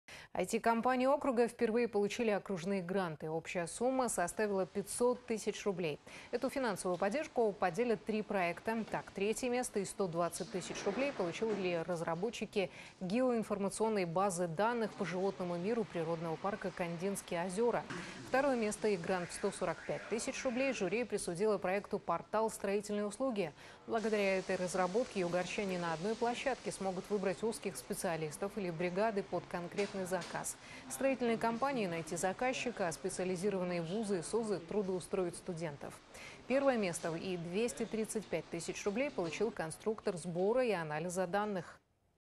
Впервые в Югре IT-проекты получили гранты
IT-компании округа впервые получили окружные гранты. Общая сумма составила 500 тысяч рублей. Эту финансовую поддержку поделят три проекта.
Третье место и 120 тысяч рублей получили разработчики геоинформационной базы данных по животному миру природного парка «Кондинские озёра».